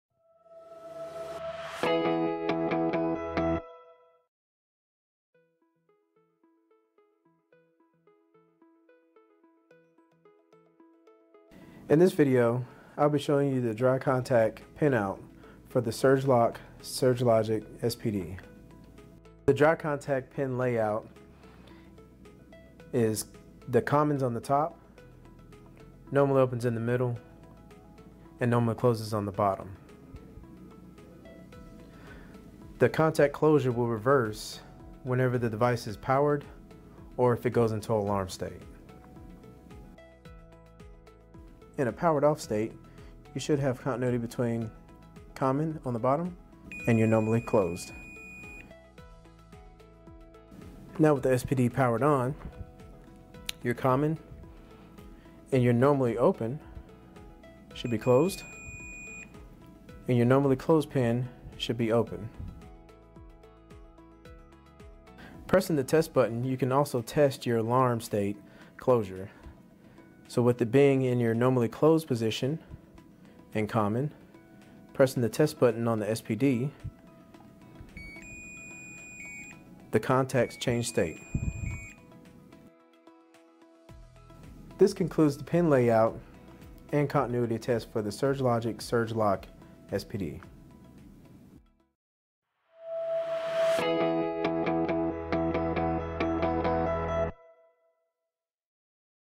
Determining Dry Contact Pinout on Surgeloc Surge Protective Device | Schneider Electric Support
Tutorial for confirming and testing the dry contact pinout (normally open, normally closed, common) on SurgeLogic Surgeloc SPD (surge protective device).